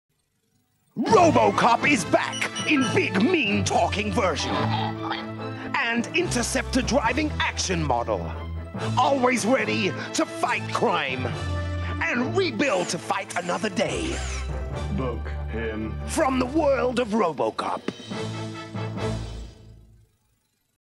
Robocop UK TV toy advert
Clipped from an off-air recording of TCC circa 1995.